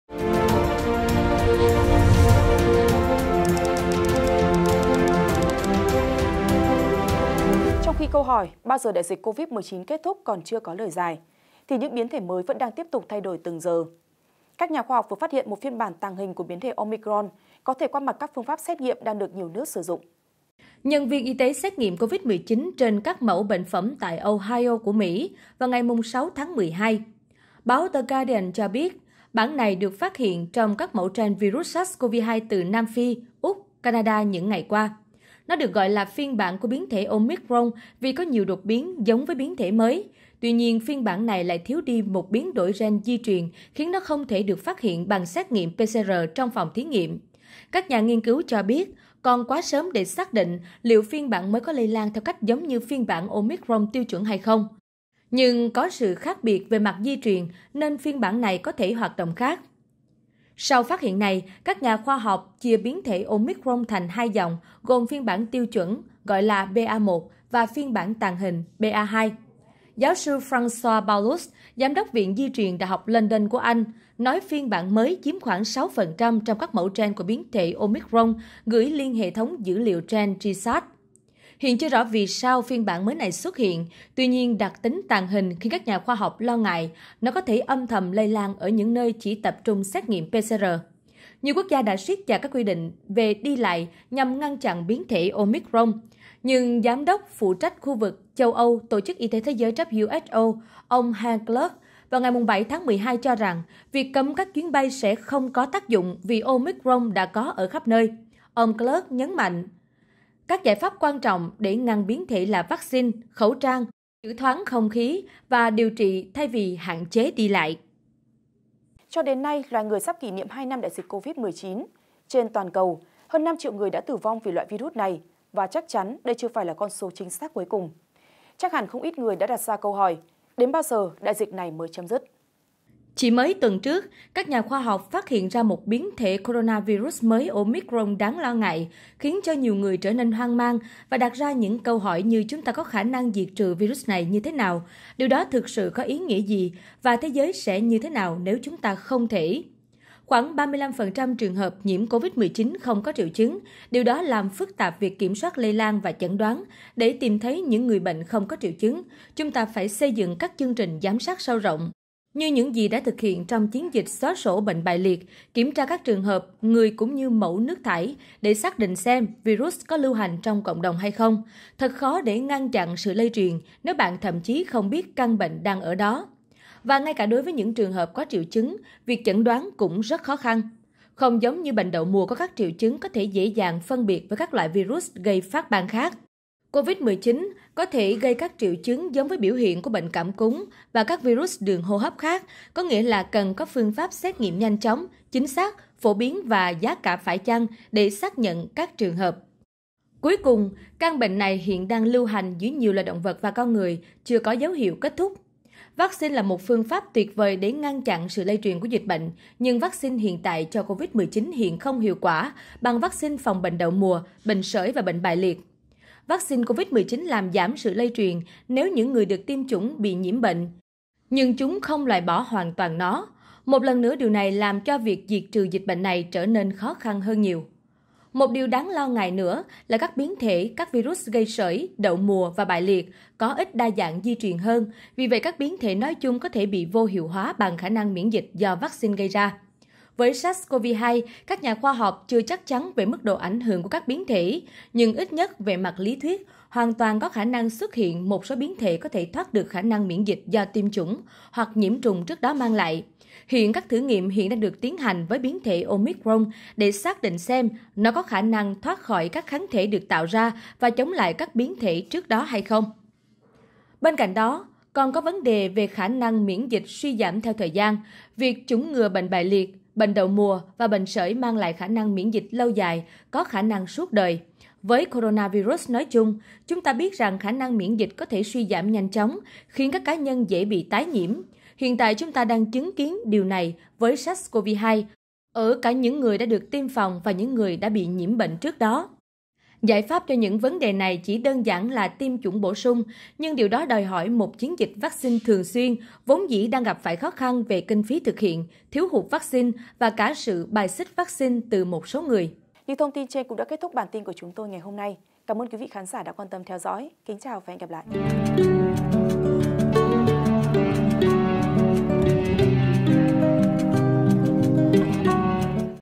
Bất Ngờ Biến Thể Omicron Có Thêm Phiên Bản "Tàng Hình" Qua Mặt Xét Nghiệm PCR | SKĐS
SKĐS | Các nhà khoa học phát hiện một phiên bản ‘tàng hình’ của biến thể Omicron có thể qua mặt các phương pháp xét nghiệm đang được nhiều nước sử dụng.

Mời quý vị và các bạn xem thêm: Hiệu Quả Vaccine Mũi 3 Tăng Hàng Chục Lần Nếu Tiêm Moderna Hoặc Pfizer? | SKĐS